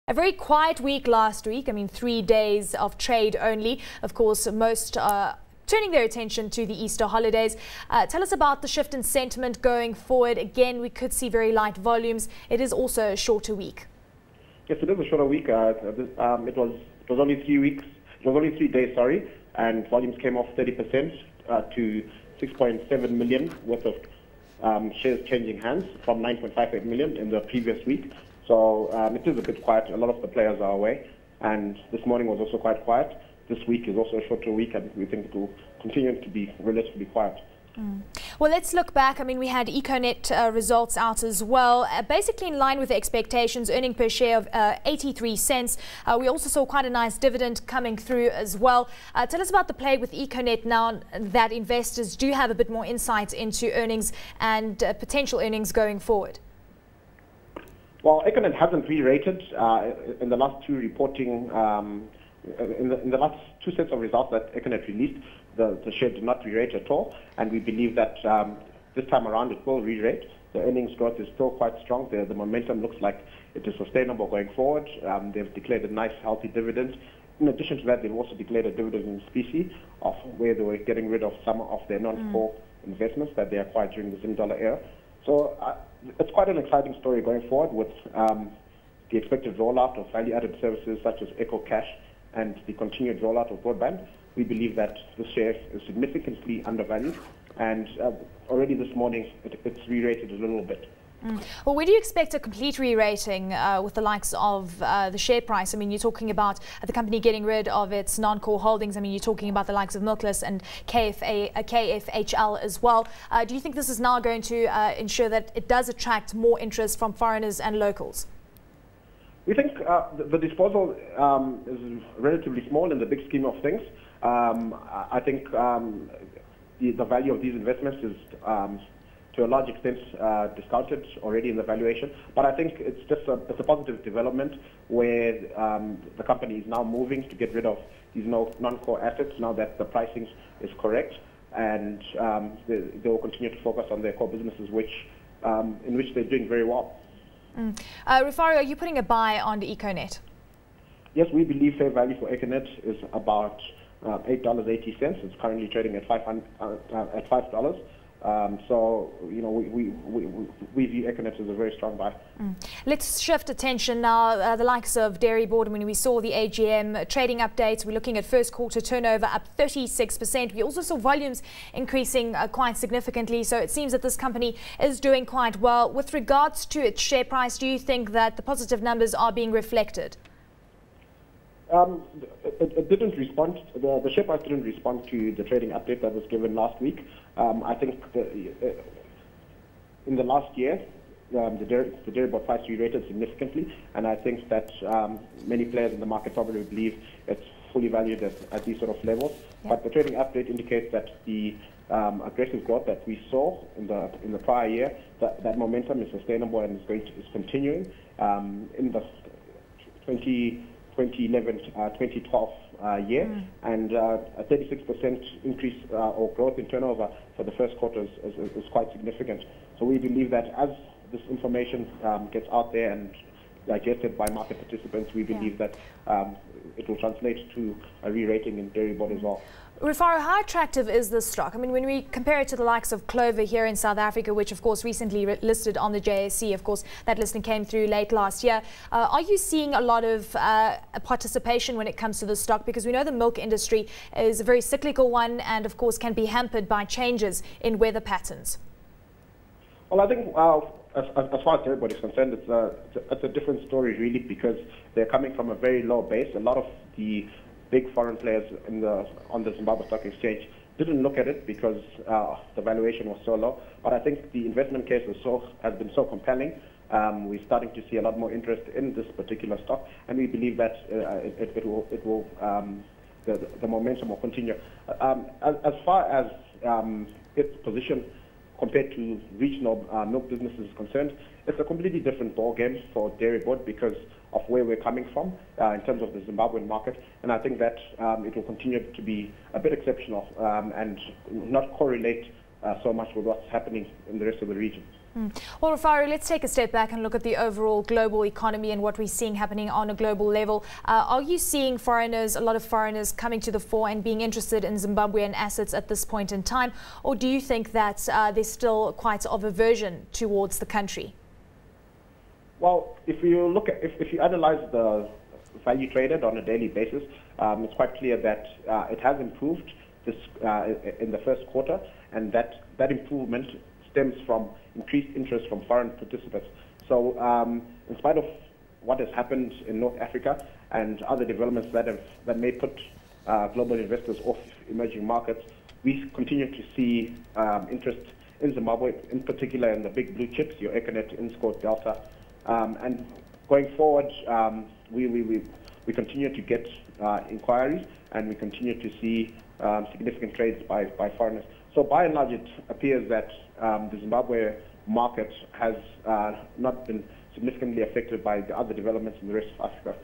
26 April - Zimbabwe Markets Update with Rufaro Zengeni
Eleni Giokos speaks with Rufaro Zengeni, Executive Director of Interfin Securities, looking at the latest financial news driving the Zimbabwe Markets.